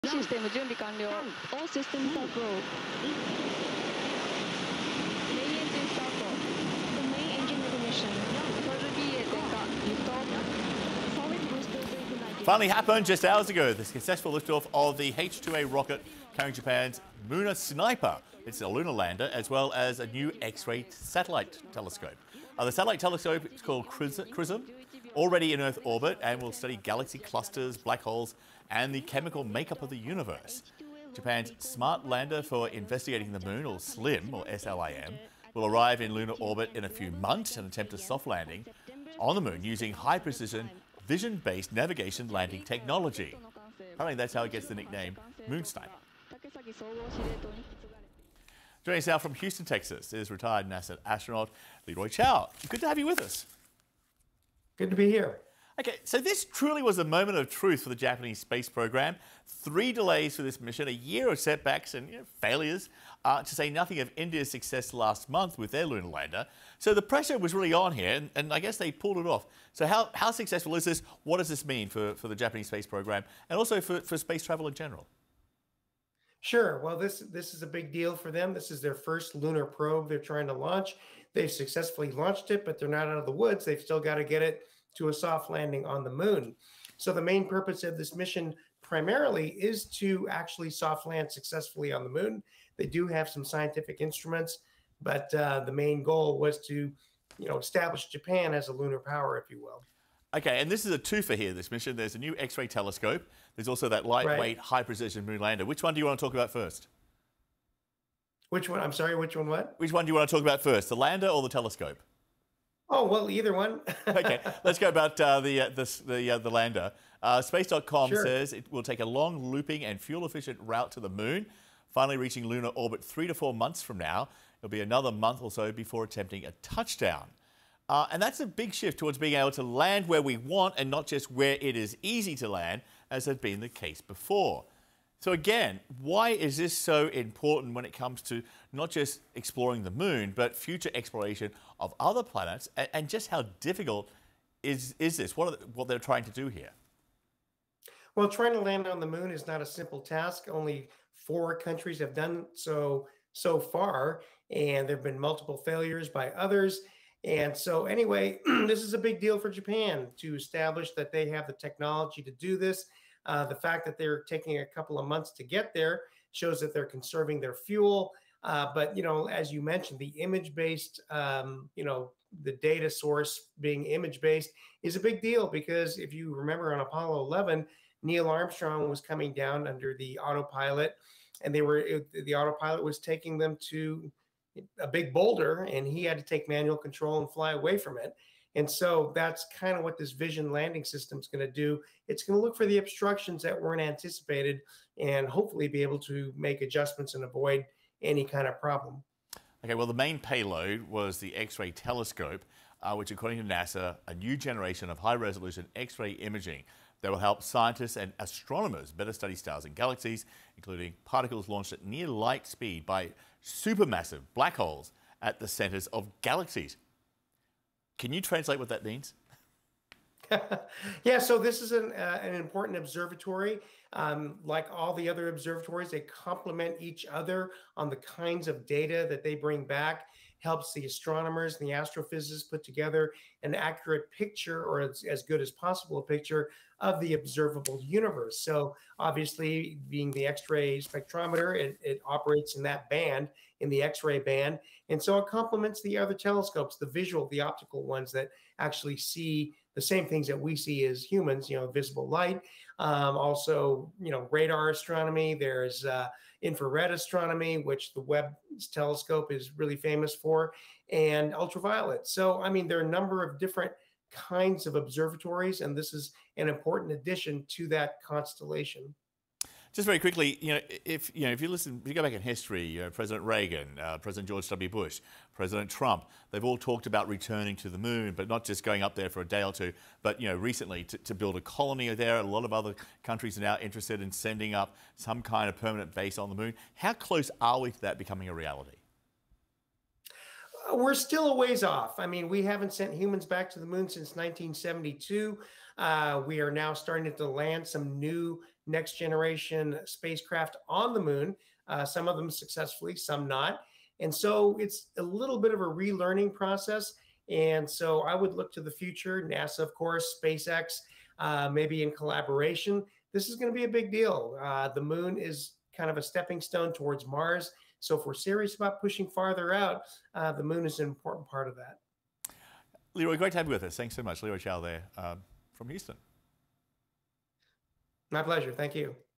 Japan launches X-ray satellite, ‘Moon Sniper’ lunar lander
CNN's John Vause talks to Former U.S. astronaut Leroy Chiao about Japan's successful launch of a revolutionary satellite that will reveal celestial objects in a new light and the “Moon Sniper” lunar lander.